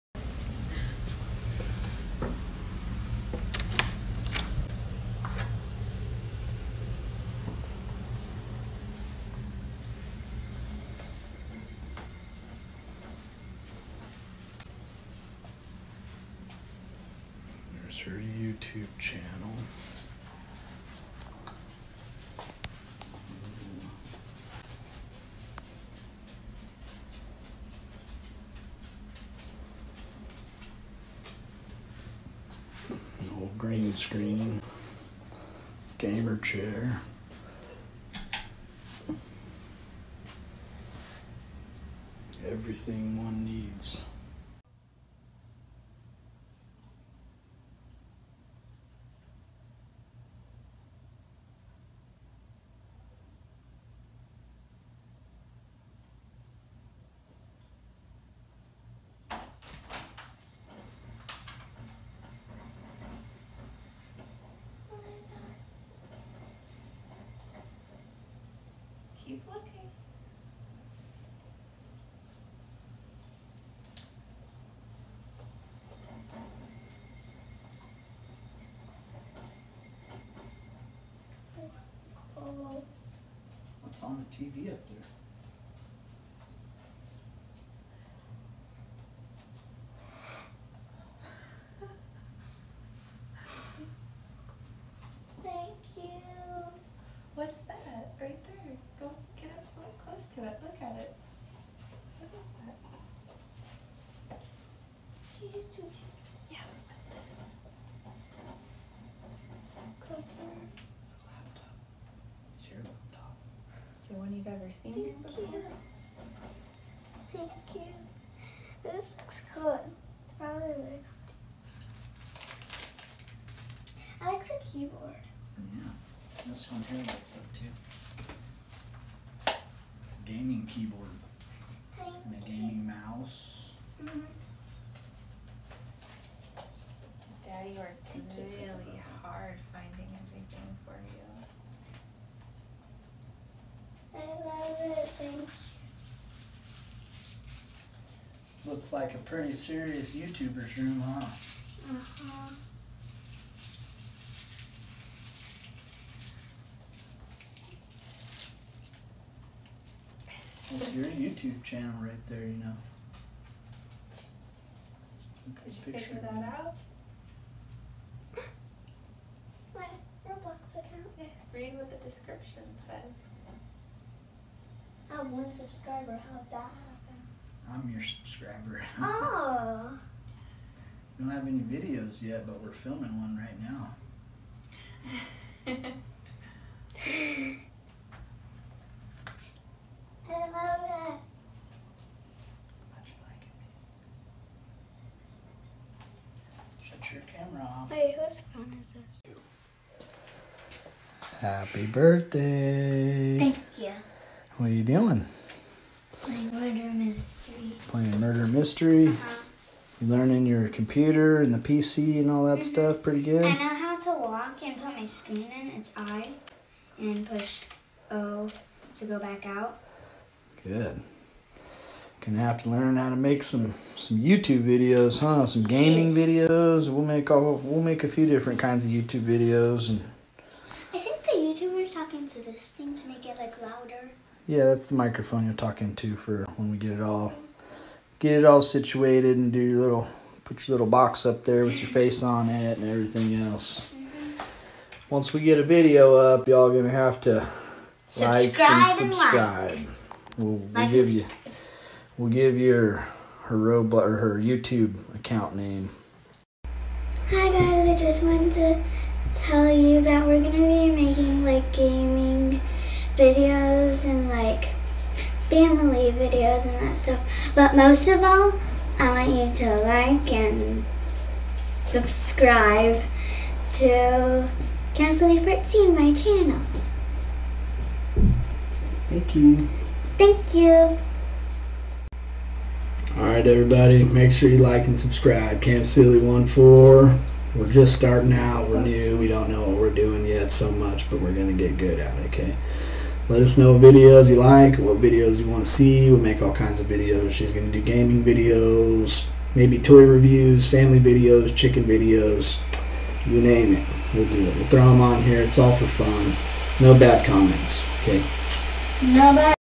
Camsilly14 Gets YouTube / Gaming Room For 7th Birthday!!!
This Is Cambrias Very First YouTube Video. Mom & Dad Transformed Her Their Office Into A YouTuber / Gaming Room For Cambria To Play Her Favorite Games & Make Fun YouTube Videos For All To Enjoy! She'll Be Making Family Fun & Gaming Videos. Roblox & Minecraft Gaming Videos To Start But Plenty More To Come Down The Road. Future Gamer Girl!